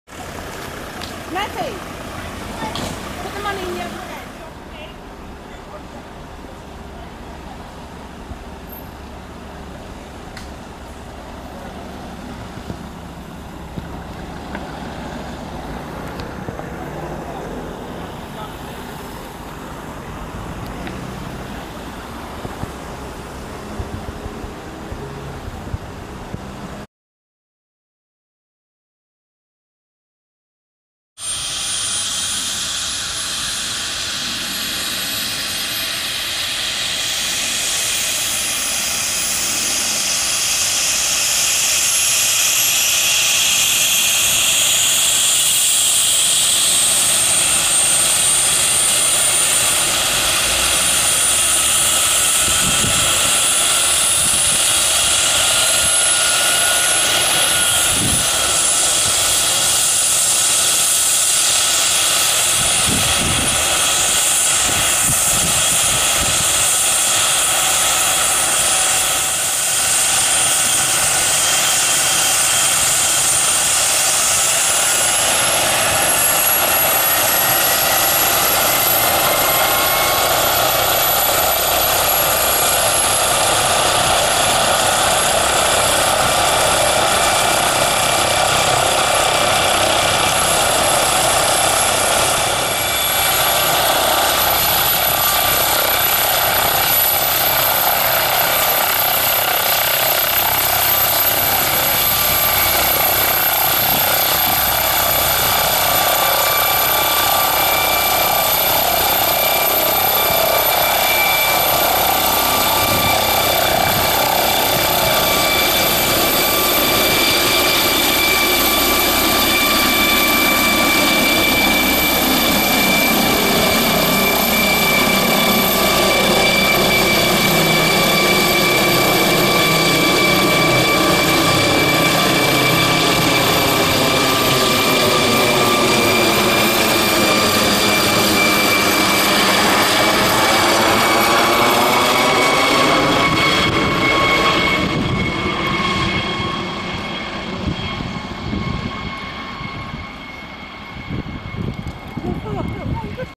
London bus crash in Manor Park - Onscene + Air Ambulance taking off
Emergency services responded to a serious collision in Manor Park, East London after a bus collided with a tree. 12 people were treated onscene by the London Ambulance Service and 10 were taken to hospital. Police and fire crews were also on scene along with the air ambulance advanced trauma team.